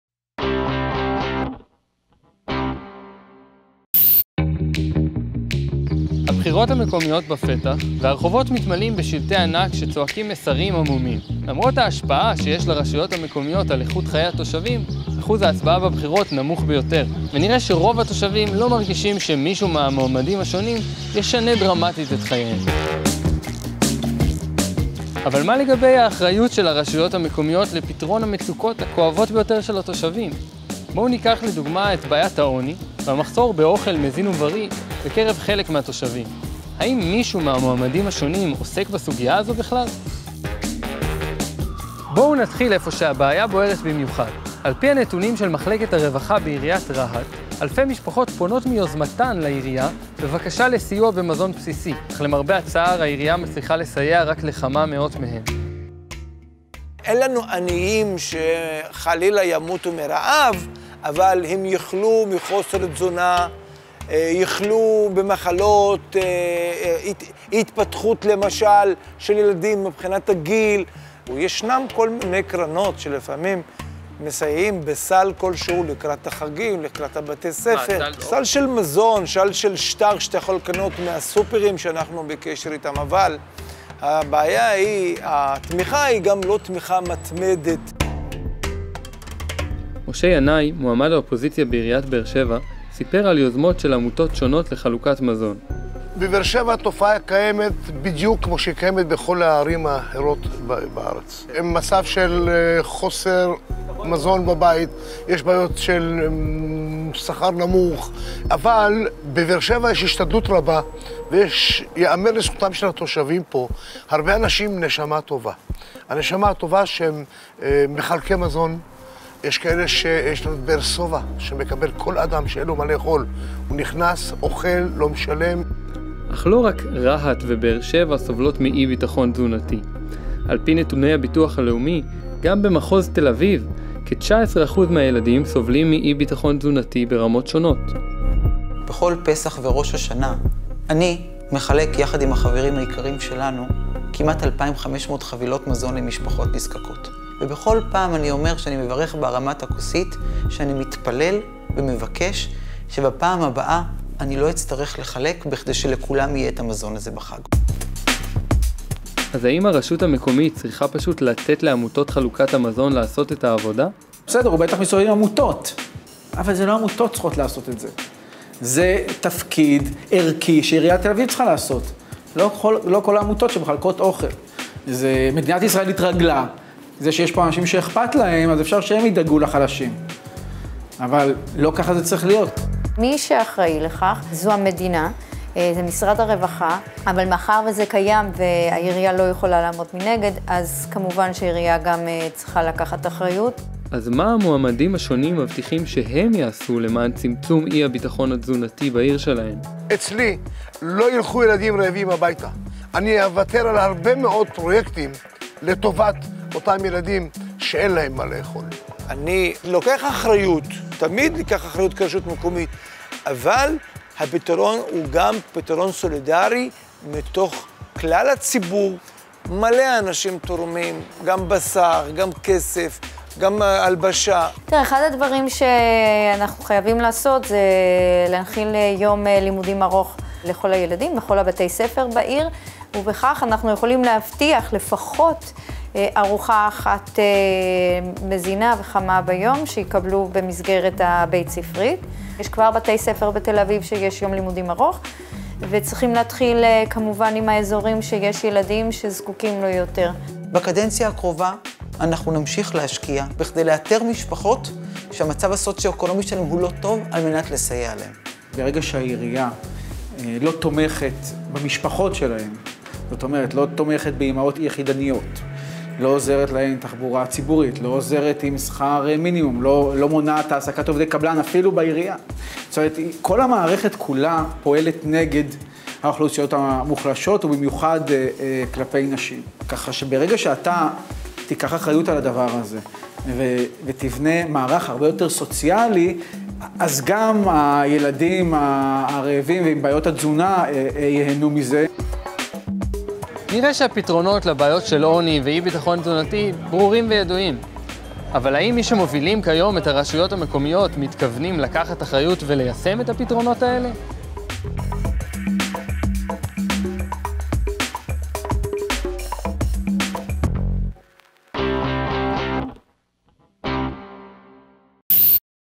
האם הרשות המקומית יכולה לצמצם אי בטחון תזונתי?
הבחירות המקומיות בפתח ומעלות את שאלת האחריות של הרשויות המקומיות לפתרון מצוקות התושבים. האם מישהו מהמועמדים השונים עוסק בסוגיית העוני והמחסור באוכל מזין ובריא, בקרב חלק מהתושבים?